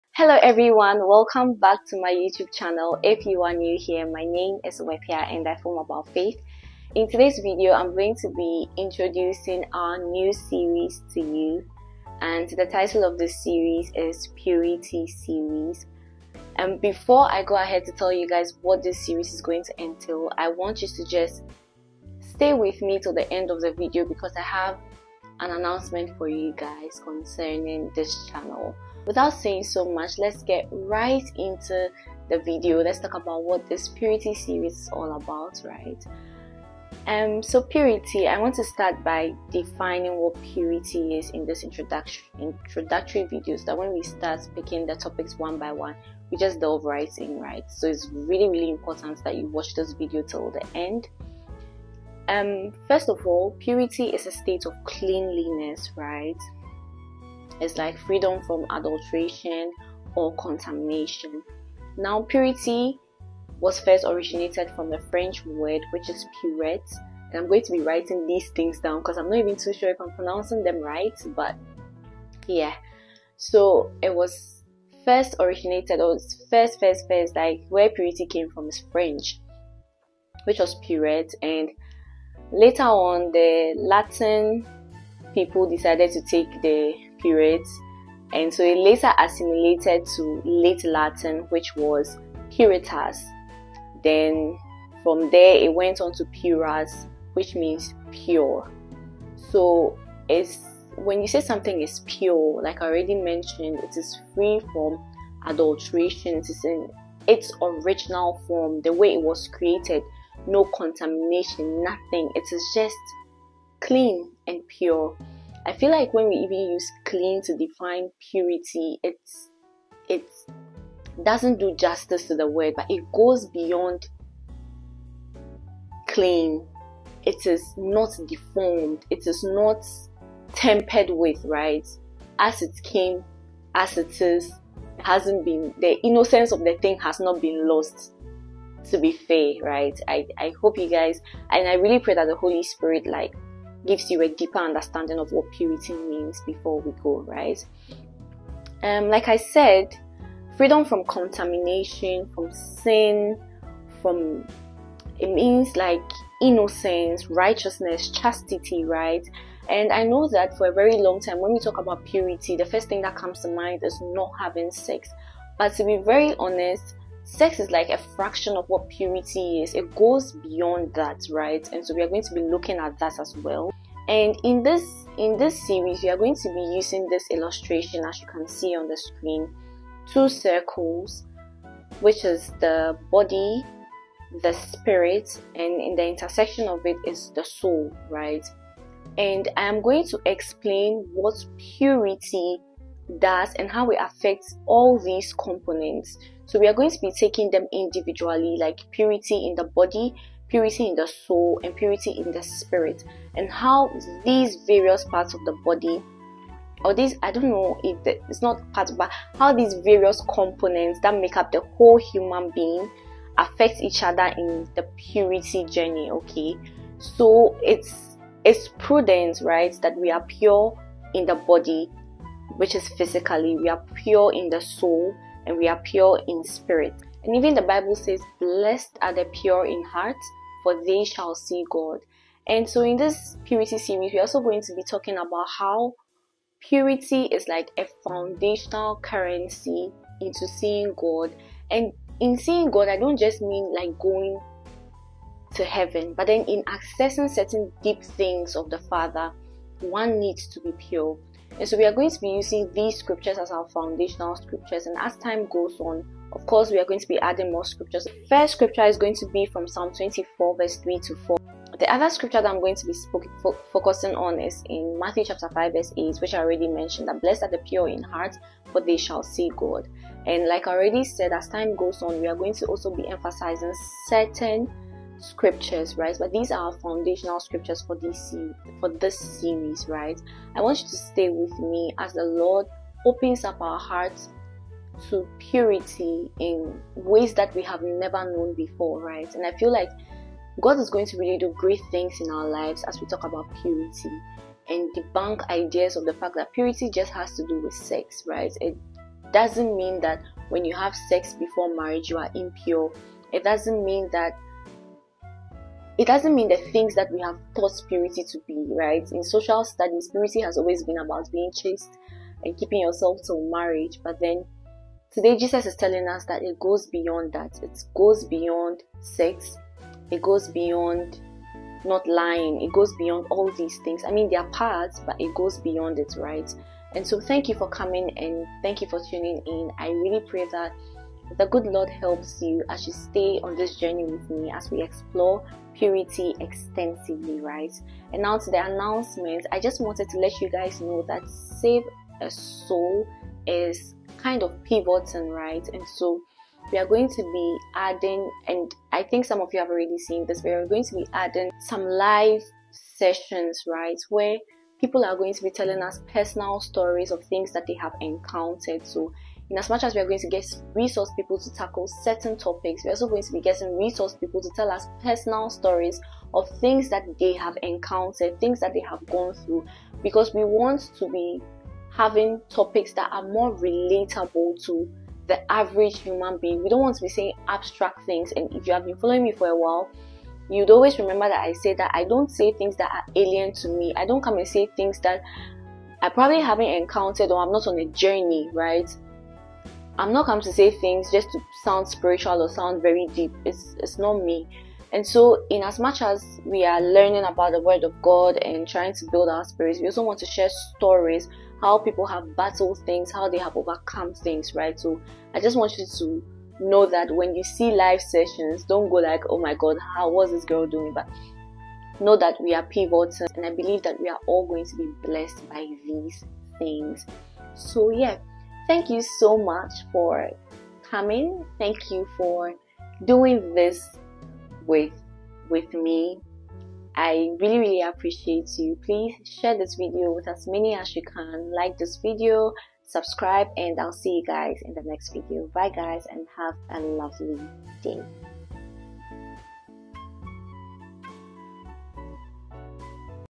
PURITY SERIES 🤍🕊️-INTRO
The Lord wants us to tackle the issue of purity and how it affects us as human beings. I just introduced the series to you and I hope you like it